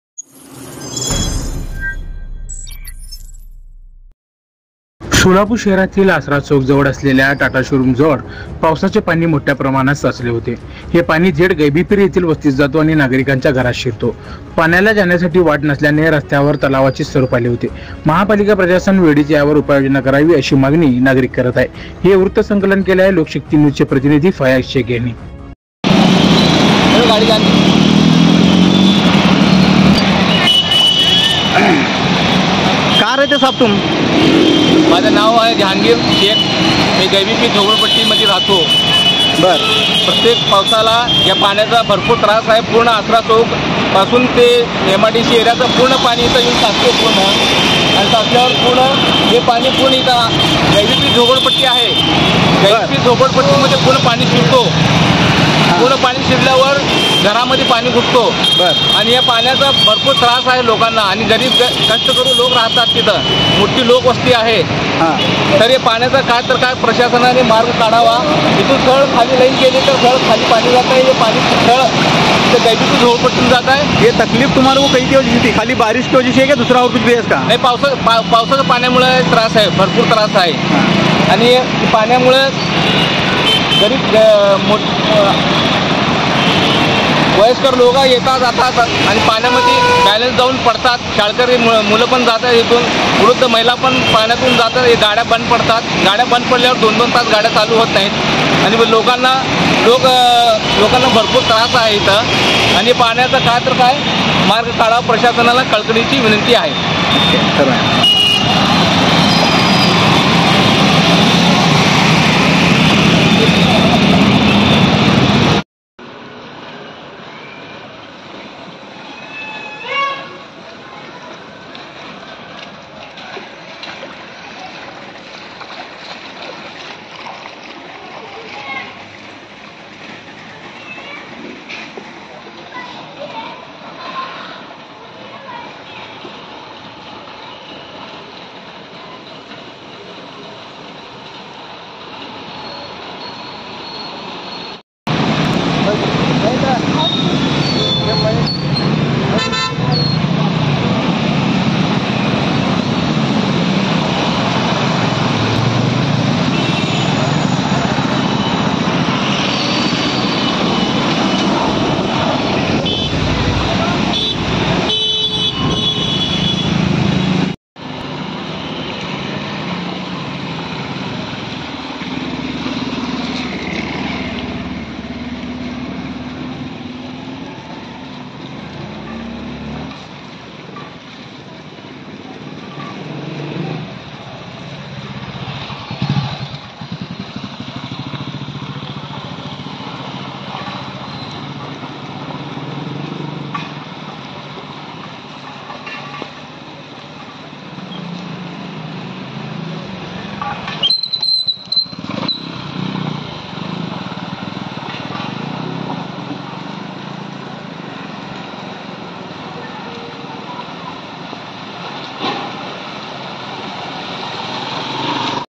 सोलापूर : होटगी रोड टाटा शोरुम समोरील रस्ता पाण्याखाली ; परिसरातील घरा मध्ये पाणीच पाणी..
Lokshakti News